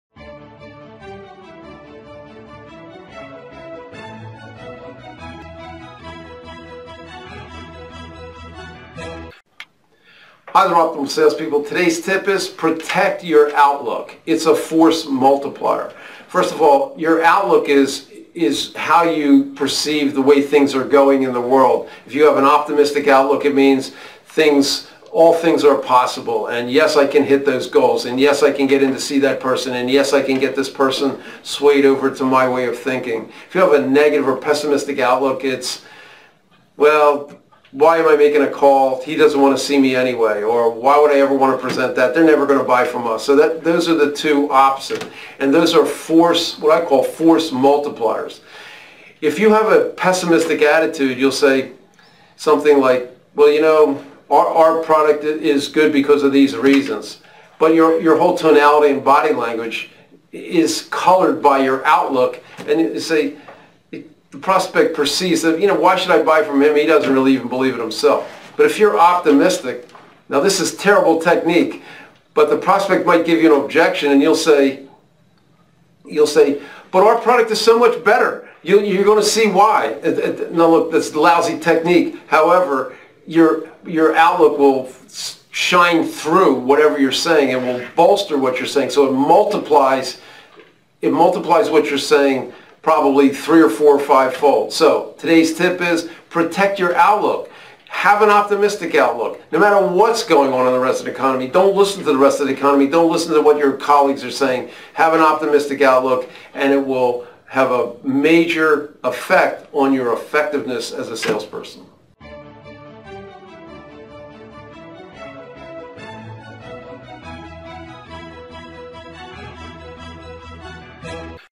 Protect Your Outlook
Protect your outlook. Be optimistic. to see more tips about how to close sales. You are driving down a highway and you make a wrong turn.  A sense of panic sets in.  You are lost!  Horrible consequences are going to occur!  You stop thinking confidently and soon you are making one mistaken turn after another and you end up more lost than you originally were.  However, if you are confident and optimistic you say to yourself - "I'll pull over to the side of the road.  I can figure this out. I'll be back on course in minutes."  You look at your GPS, figure out the best route to recover, and you arrive at your destination only a few minutes later.  Your outlook has everything to do with the actions you subsequently take, actions that multiply and either make things much better or make things much worse.  What is your outlook regarding your sales?  Are you optimistic that client's will purchase your solution?  Or, are you pessimistic, conveying to your prospect through tone and body language that you are defeated?  Protect your outlook.  Your outlook is a multiplier.